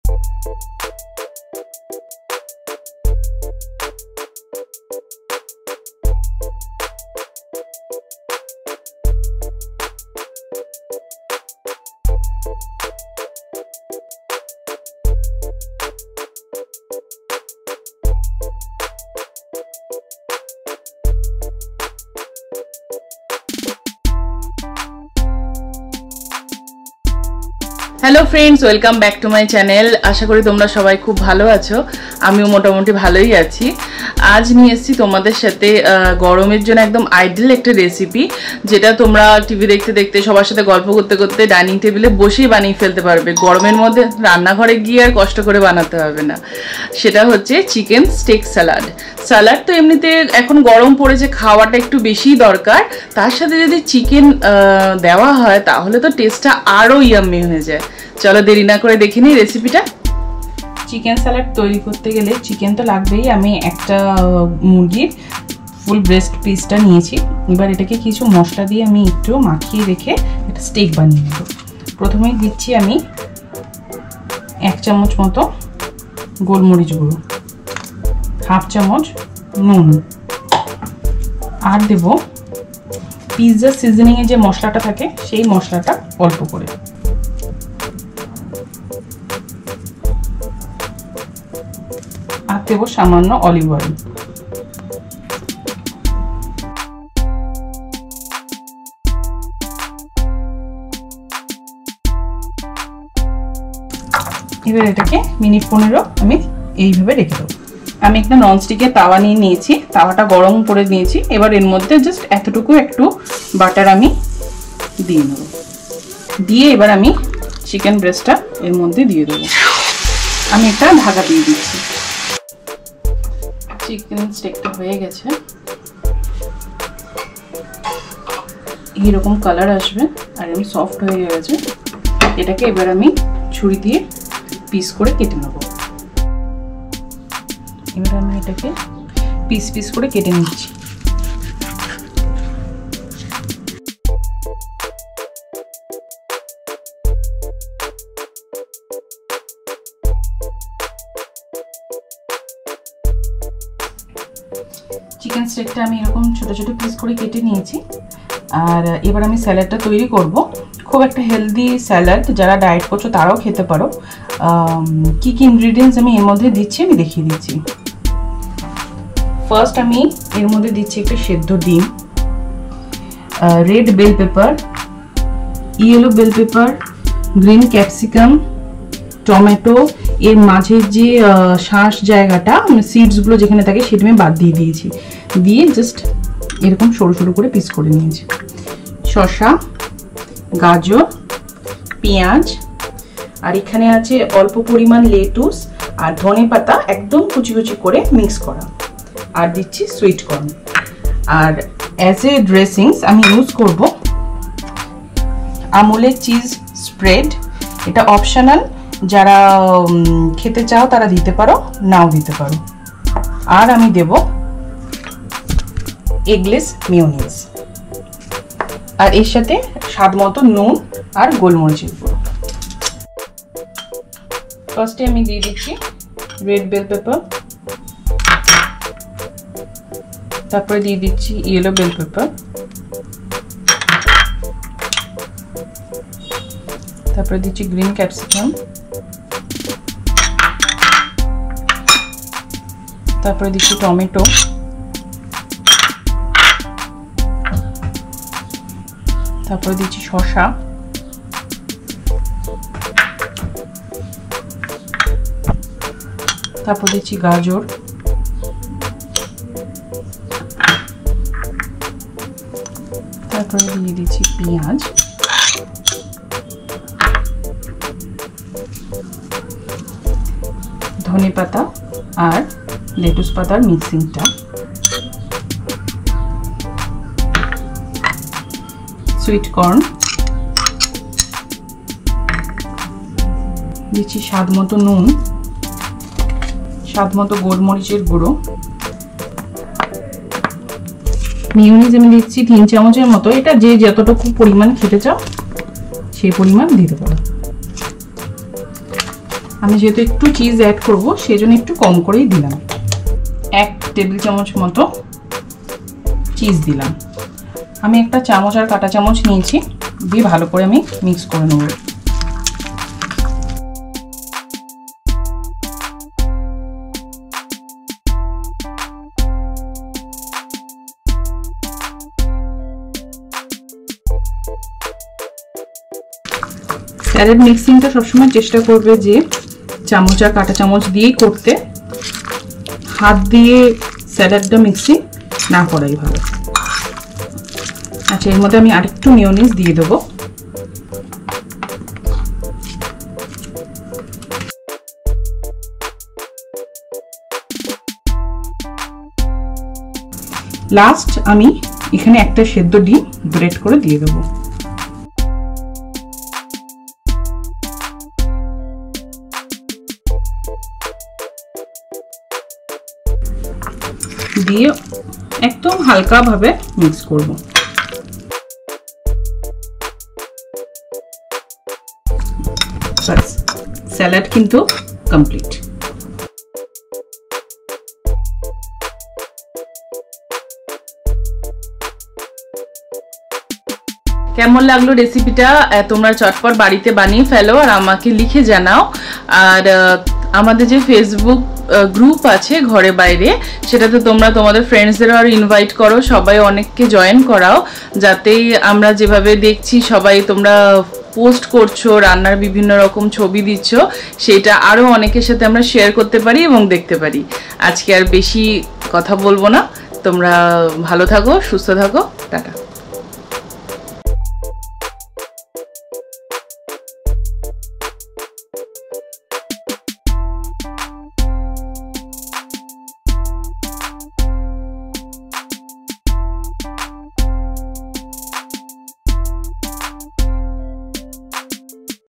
Chicken steak salad recipe at home
Ingredients:

1. Chicken breast.
2. Capcicum.
3. Red bell pepper.
4. Yellow bell pepper.
5. Cucumber.
6. Carrot.
7. Onion.
8. Sweet corn.
9. Boiled egg.
10. Lettuce and Coriander leafs.
11. Mayonnaise.
12. Cheese spread (Optional).
13. Olive oil.
14. Salt as per taste.
15. Black pepper powder.